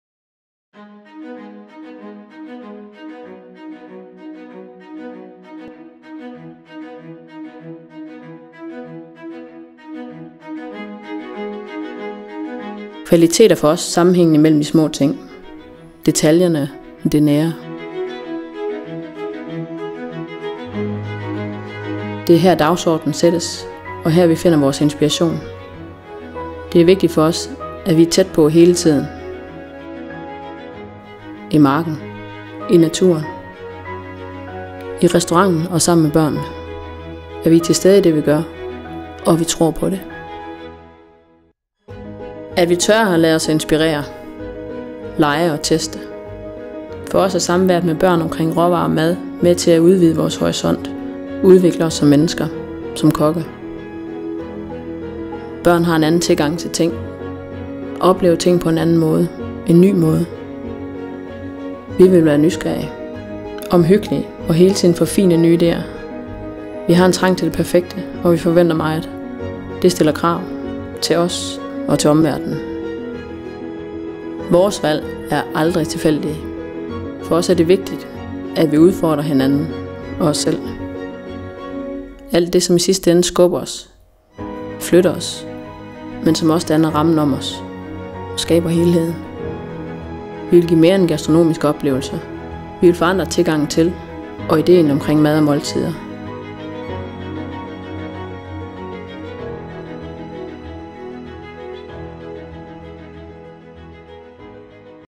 Kentaur presents the michelin restaurant Ti Trin Ned
Mette and Rainer Gassner from the Michelin restaurant Ti Trin Ned, Fredericia have always had good ideas and contributed  to our product development as well as testing our novelties,  when it goes intensely in the kitchen. Now you can meet them here 😊